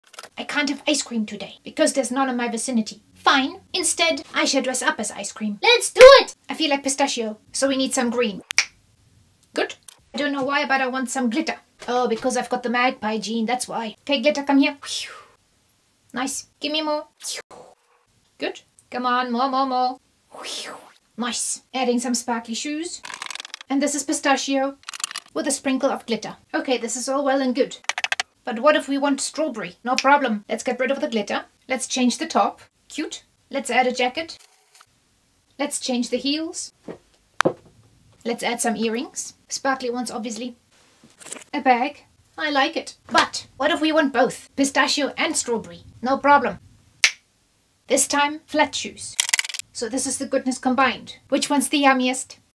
When you decide to dress up as ice cream 🍦
You asked me to style some bright summer outfits so here are some options.